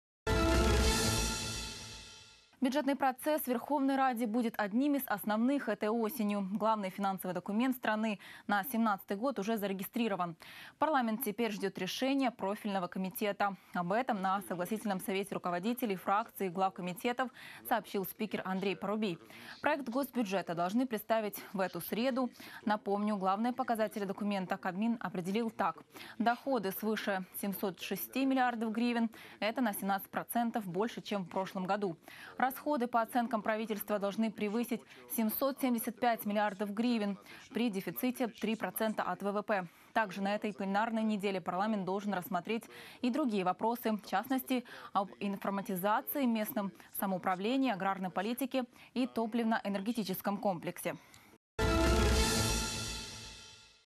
Рассмотрят проект госбюджета
Бюджетный процесс в Верховной Раде будет одним из основных  этой осенью. Главный финансовый документ страны на 2017 год уже зарегистрирован.